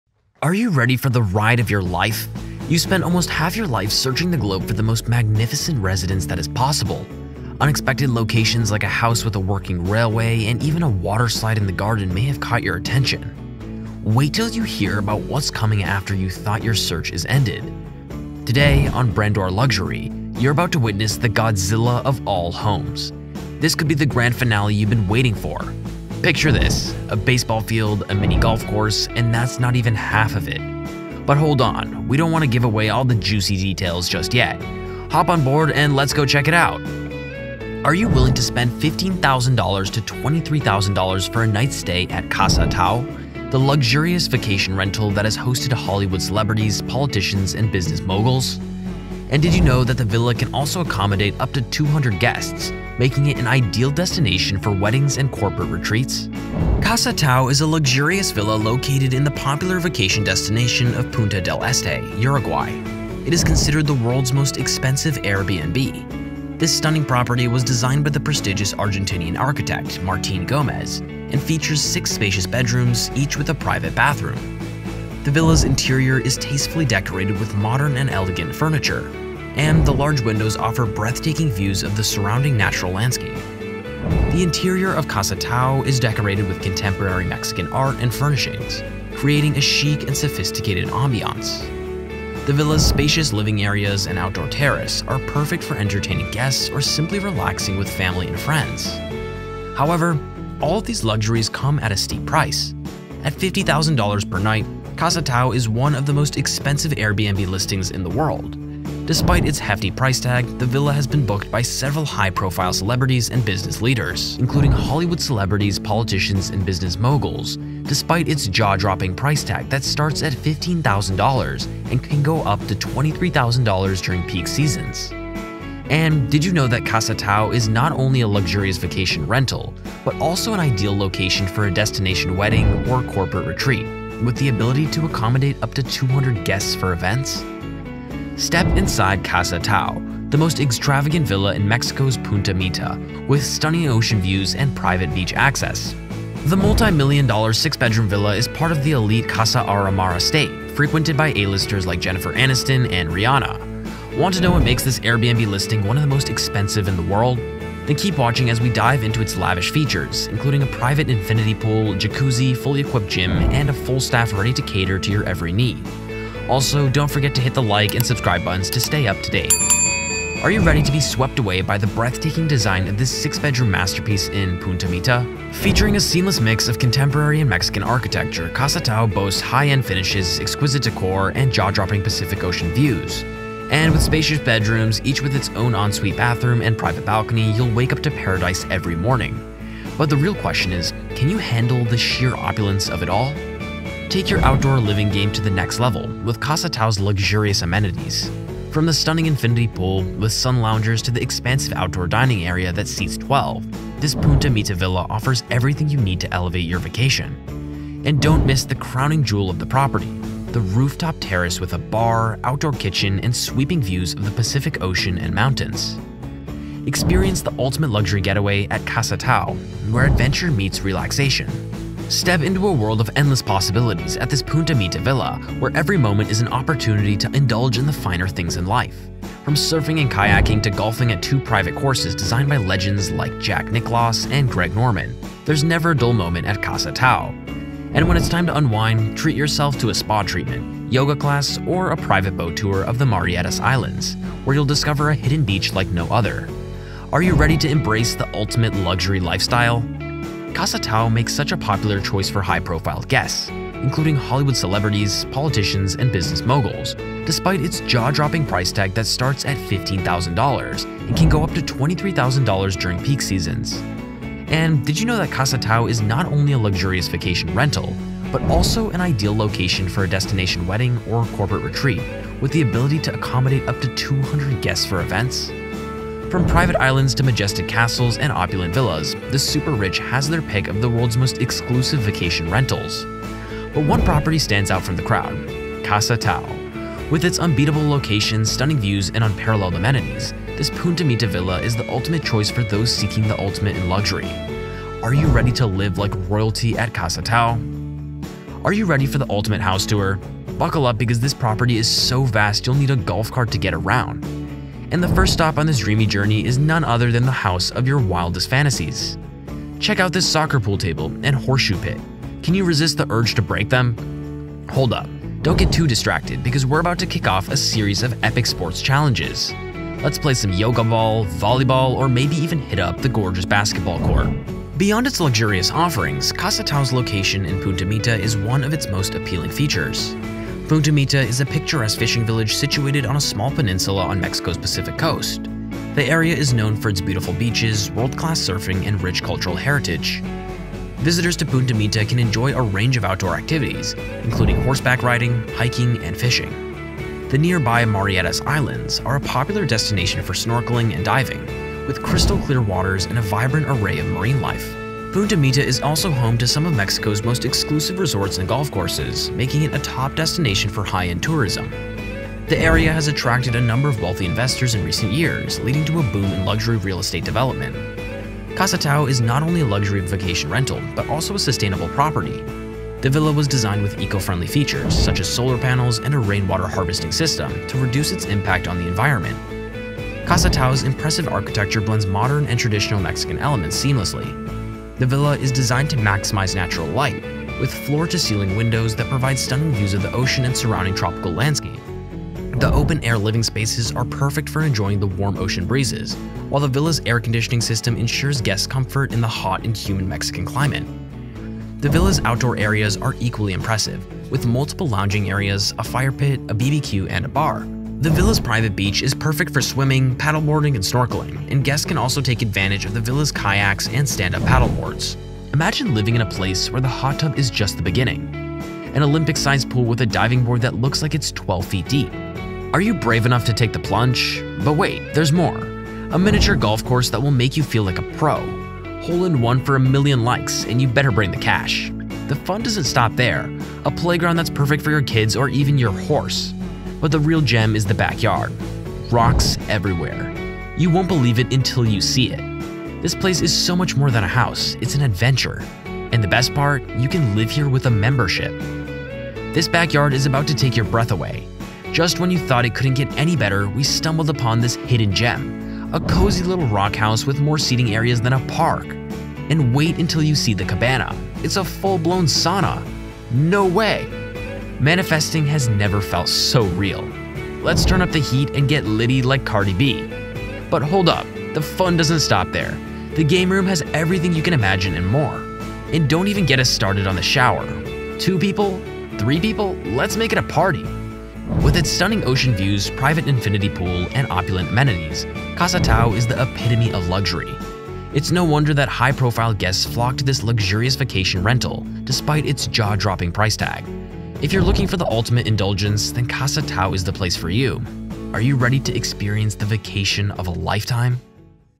Where Is The World's Most Expensive AirBnB?
"Casa Tau" is not just any ordinary Airbnb, it's the world's most expensive one. Located in the luxurious beachfront town of Punta del Este, Uruguay, this stunning property boasts eight spacious bedrooms, nine bathrooms, and an impressive list of amenities, including a private pool, gym, cinema, and even a helipad. Designed by renowned architect Rafael Viñoly, Casa Tau features sleek modern architecture and breathtaking ocean views. This exclusive retreat can accommodate up to 16 guests and comes with a full staff to ensure a luxurious and unforgettable experience. With a nightly rate of $10,000, Casa Tau is truly a once-in-a-lifetime opportunity to live like royalty on your next vacation.